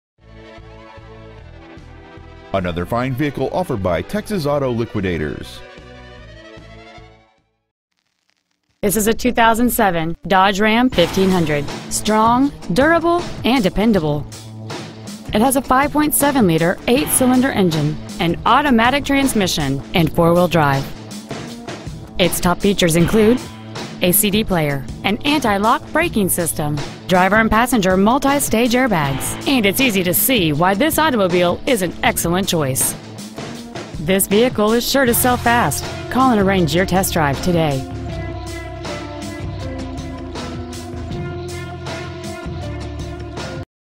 2007 Dodge Ram 1500 Arlington TX
Year: 2007
 Make: Dodge
 Model: Ram 1500
 Engine: 5.7L V8 HEMI MULTI-DISPLACEMENT ENGINE
 Trans.: Automatic
 Exterior: Silver
 Miles: 83,711
 Interior: Gray

 Pre-Owned 2007 Dodge Ram 1500 Arlington TX